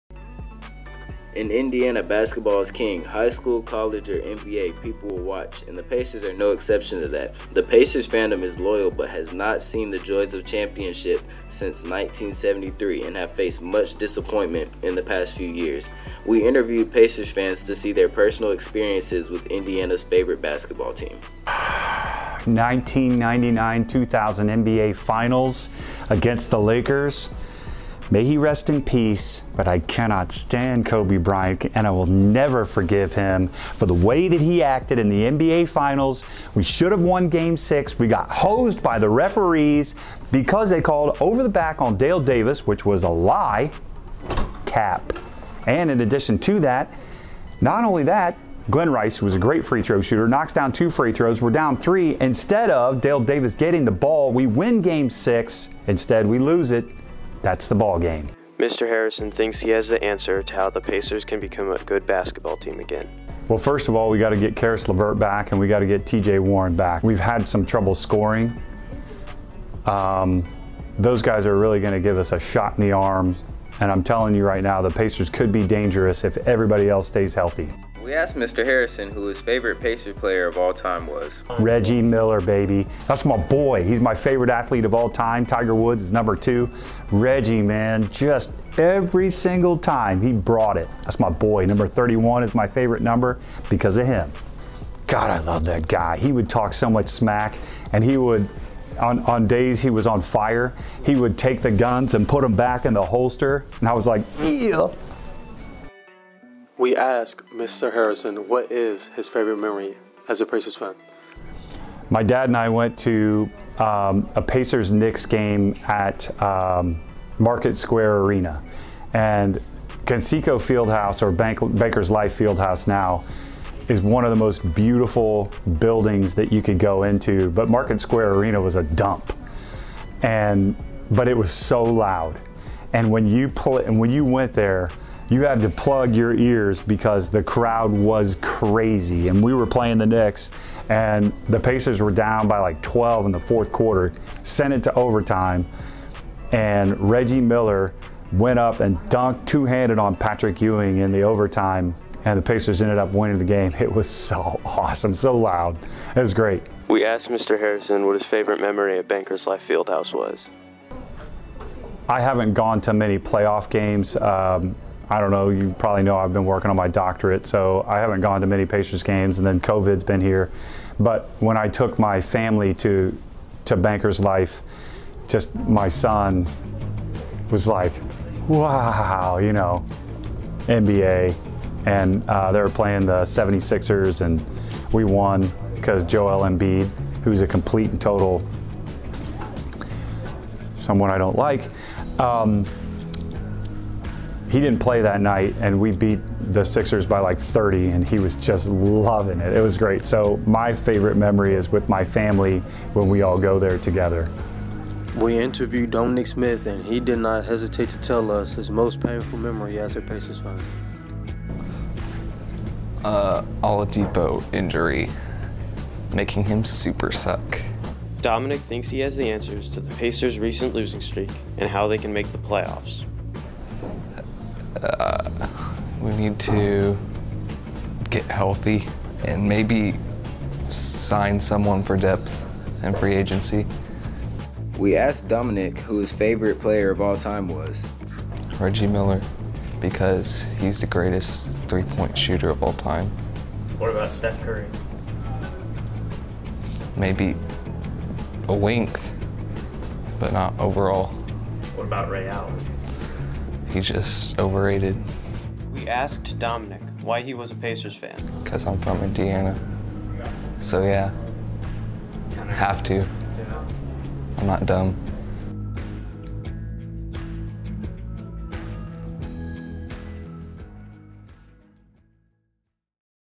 Life As A Pacer Fan
Being a Pacers fan has been tough over the year. WHEN explores why, and what fans think the answer might be.

Credits: Luc Wineland-Moening, Ty Burdine, and Cam Harris